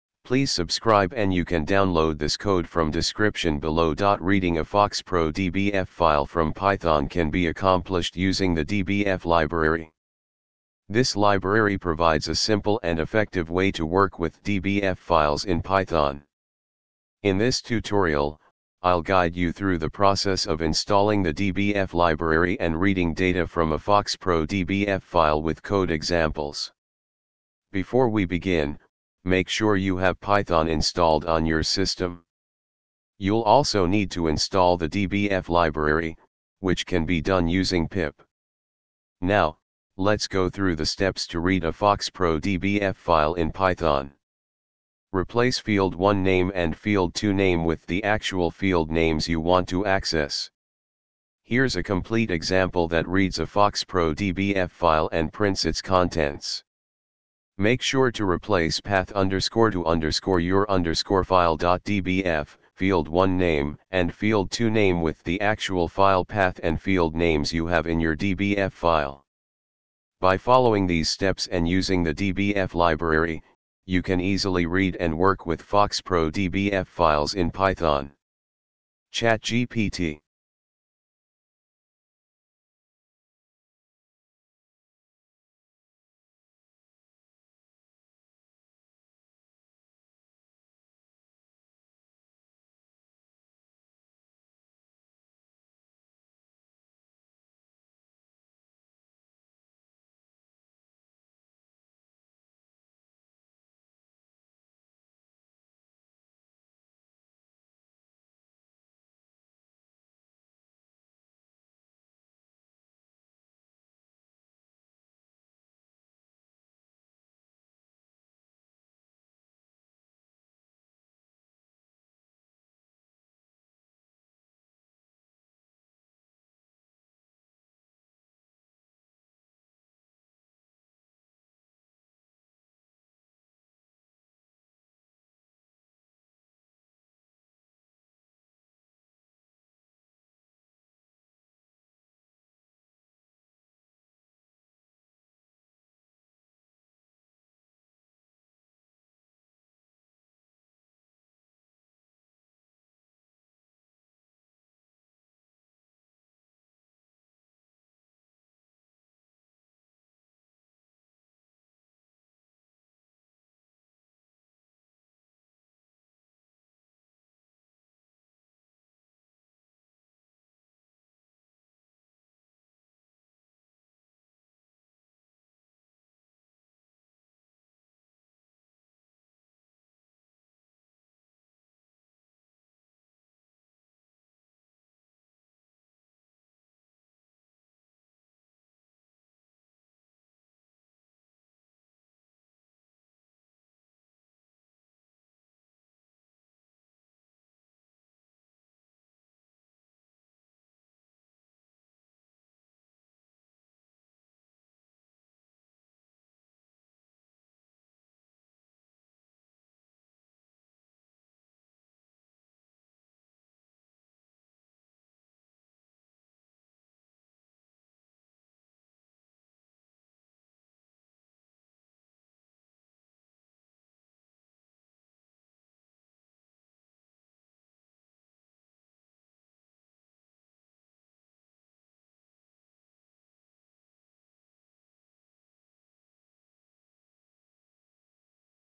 What's the easiest way to read a FoxPro DBF file from Python
reading a foxpro dbf file from python can be accomplished using the dbf library. this library provides a simple and effective way to work with dbf files in python. in this tutorial, i'll guide you through the process of installing the dbf library and reading data from a foxpro dbf file with code examples.
before we begin, make sure you have python installed on your system. you'll also need to install the dbf library, which can be done using pip:
now, let's go through the steps to read a foxpro dbf file in python.
replace 'field1name' and 'field2name' with the actual field names you want to access.
here's a complete example that reads a foxpro dbf file and prints its contents:
make sure to replace 'path_to_your_file.dbf', 'field1name', and 'field2name' with the actual file path and field names you have in your dbf file.
by following these steps and using the dbf library, you can easily read and work with foxpro dbf files in python.